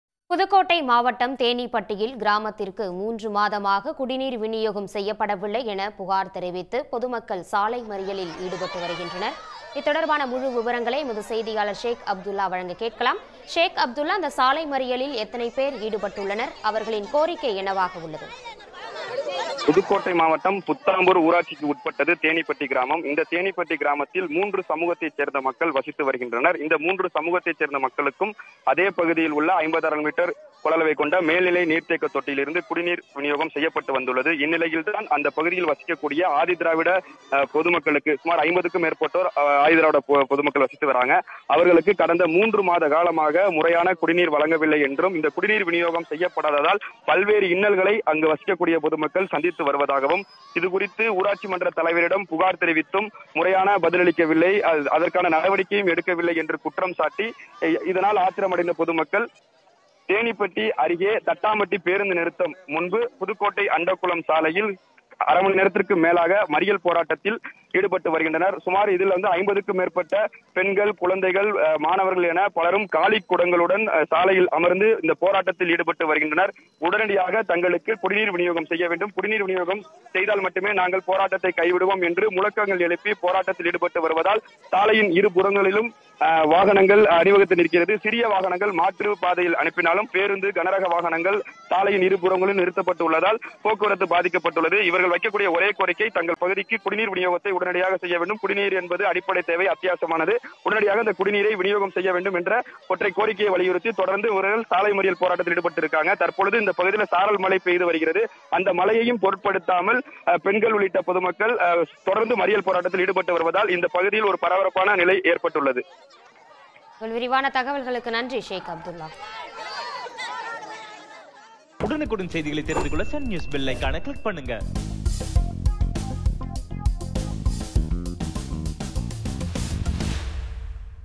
People Protest For water : Shock in pudukkottai | Sun News | Just in
குடிநீர் விநியோகம் நிறுத்தம்...3 மாதமாக நடக்கும் போராட்டம்..கொதித்தெழுந்த மக்கள்!